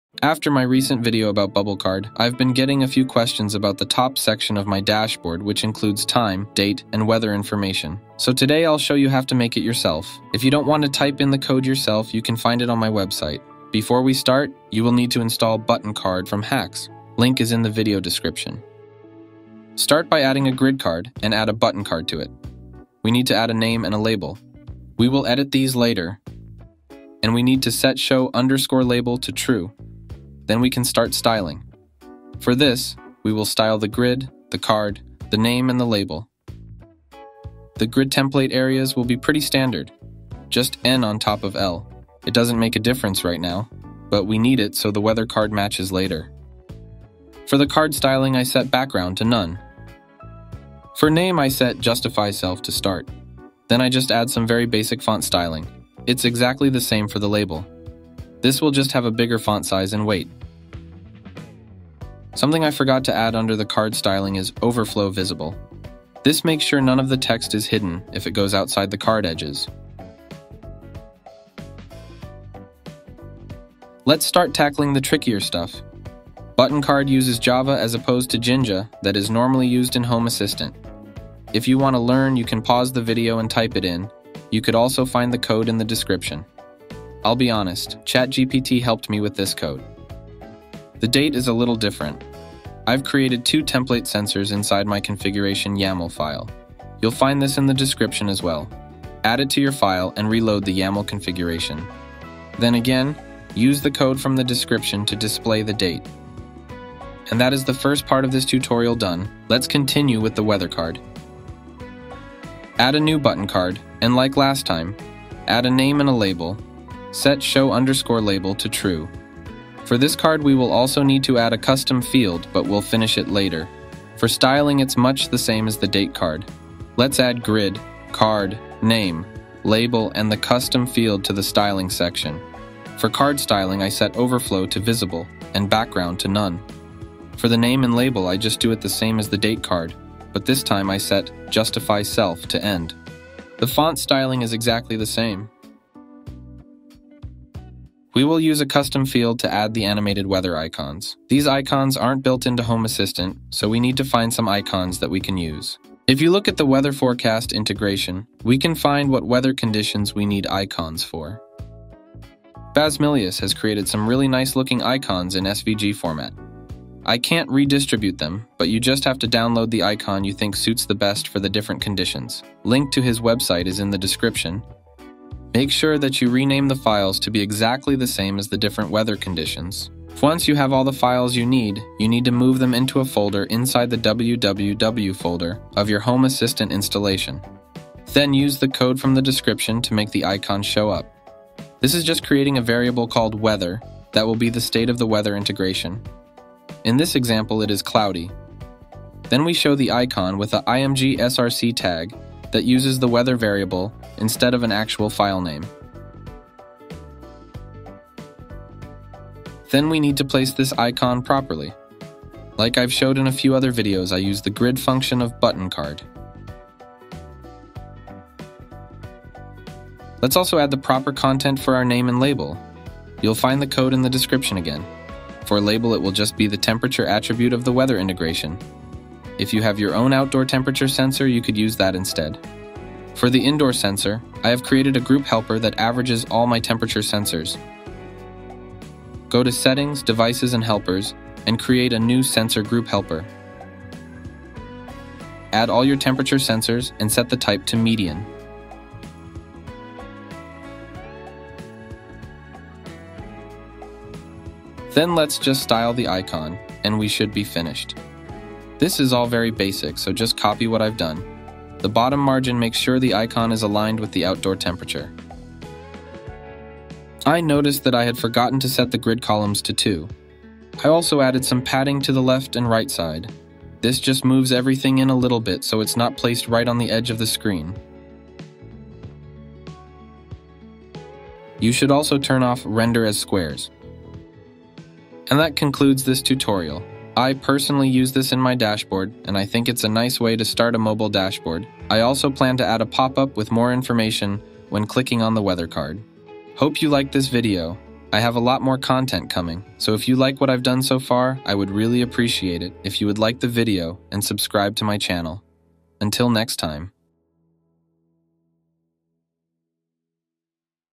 Animated Weather Card in Home Assistant
In this video I will show you how I have created the top section of my dashboard where I have time, date and weather information.